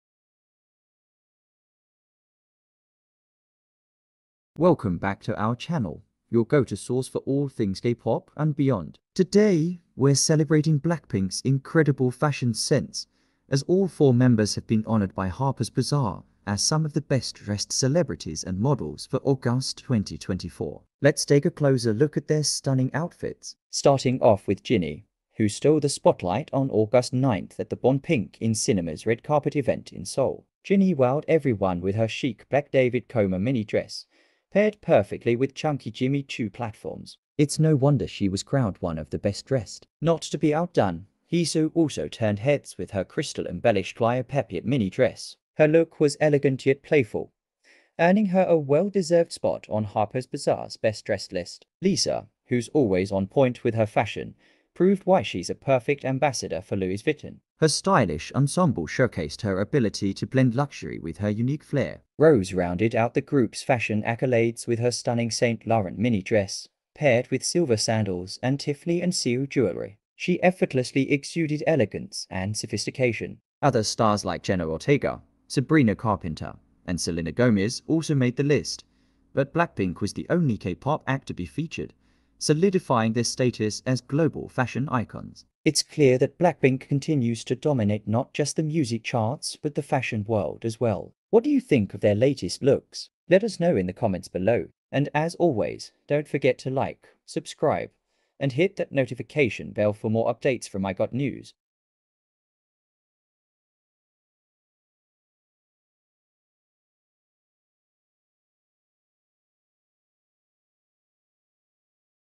BLACKPINK Dominates Harper’s Bazaar: Best Dressed Celebrities & Models in August!
BLACKPINK is not just dominating the music charts; they’re also taking over the fashion world! 🖤✨ The global superstars were named among Harper’s Bazaar’s best-dressed celebrities and models for August, showcasing their unmatched style and elegance. Whether it’s on the red carpet, at fashion events, or casual street style, the BLACKPINK members continue to set trends and inspire fans worldwide with their iconic fashion choices. 👑📸
From Jennie’s chic ensembles to Rosé’s effortlessly cool looks, Lisa’s edgy style, and Jisoo’s elegant outfits, the group proves why they are true fashion icons. 🌟🖤 In this video, we’ll break down each member’s standout looks from Harper’s Bazaar, discuss their impact on the fashion industry, and explore how BLACKPINK continues to influence global style trends. 💫
Join us as we dive into the details of BLACKPINK’s latest fashion feature, their collaborations with top designers, and why they are considered the ultimate style queens of the K-pop world. Get ready for a dose of fashion inspiration straight from your favorite idols! 💅💖